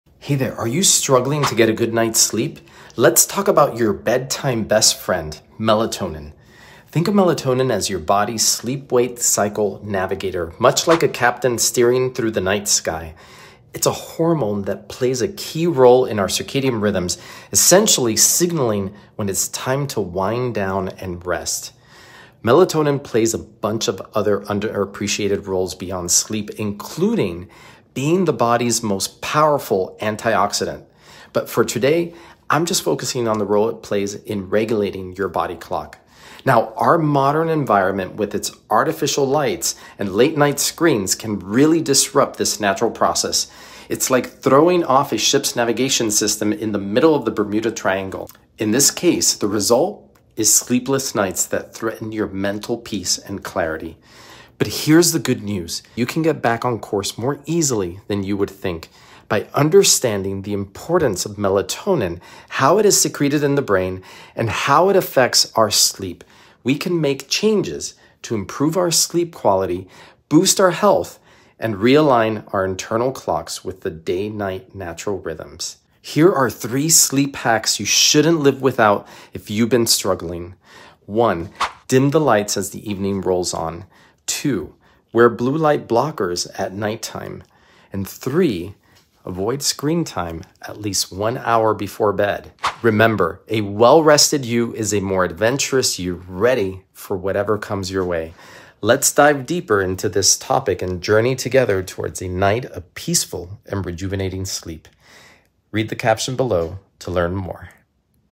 This One Hormone Can Fix Your Sleep & Health
This one hormone is your secret weapon for sleep - and you can unlock its power tonight!

Let’s embark on a trip to never-never land with melatonin, our body's natural guide to sleep and circadian rhythms.

Sleep, a crucial aspect of our overall health, is profoundly influenced by melatonin, a hormone pivotal in guiding us toward deep, restorative sleep.

There are practical steps we can take to harness the full potential of melatonin and synchronize our sleep patterns with the earth’s natural rhythms. This boosts our health and energy and enriches our overall well-being.

Dr. P